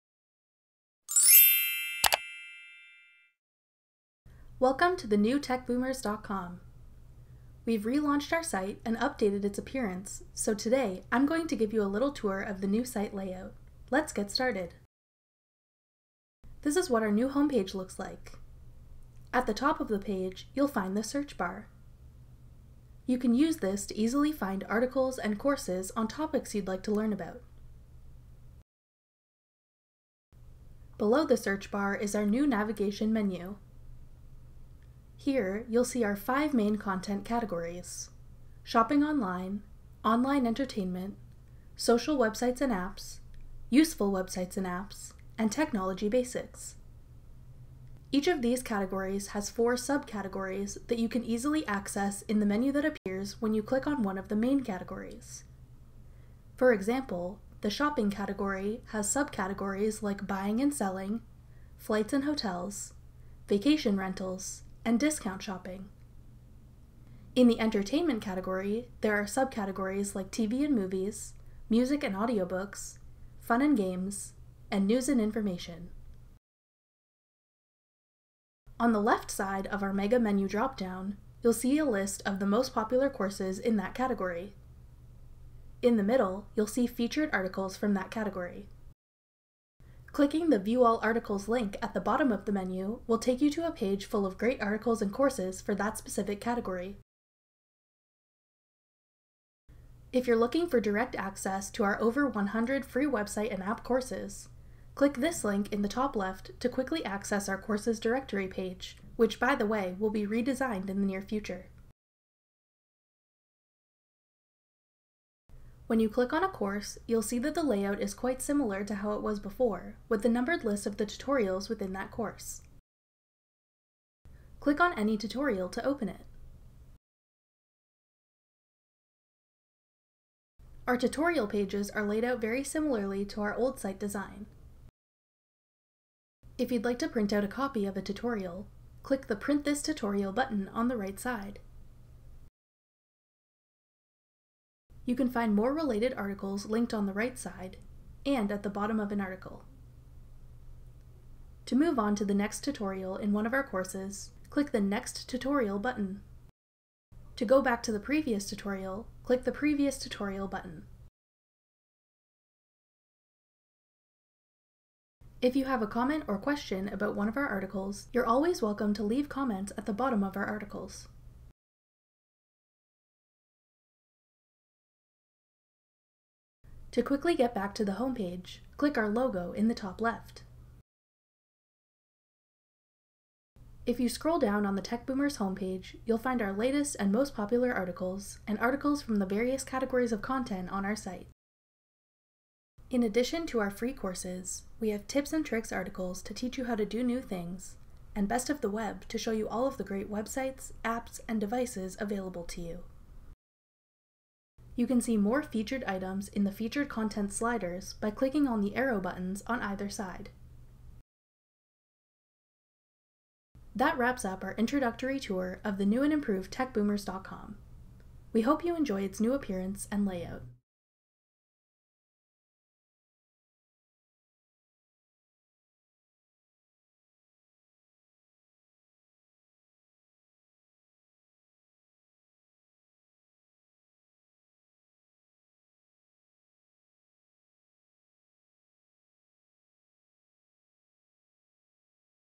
TechBoomers Website Relaunch Tour
Welcome to the new TechBoomers.com! We've relaunched our website and updated its appearance, so this video was created to give you an introductory tour. Watch this video to get a guided tour of our new site layout.

At the top of the site, you'll find our convenient search bar. Use this tool to easily find articles and courses on topics you'd like to learn about. Below the search bar is our new navigation menu. There are 5 main content categories: shopping online, online entertainment, social websites and apps, useful websites and apps, and technology basics. Each of these categories has four subcategories that you can easily access in the menu that appears when you click on one of the main categories.

On the left side of our mega menu drop-down, you’ll see a list of the most popular courses in that category. In the middle, you’ll see featured articles from that category. Clicking the View All Articles link at the bottom of the menu will take you a page full of great articles and courses for that specific category.

If you’re looking for direct access to our over 100 free website and app courses, click Access 100+ Free Courses.

The layout for our courses and tutorial pages is quite similar to our old website design. If you’d like to print out a copy of a tutorial, click the Print This Tutorial button on the right side. You can find more related articles linked on the right side, and at the bottom of an article. To move on to the next tutorial in one of our courses, click the Next tutorial button. To go back to the previous tutorial, click the Previous Tutorial button.

If you have a comment or question about one of our articles, you’re always welcome to leave comments at the bottom of our articles.

We hope you find this tour video helpful, and that you enjoy the new appearance and layout of TechBoomers.com.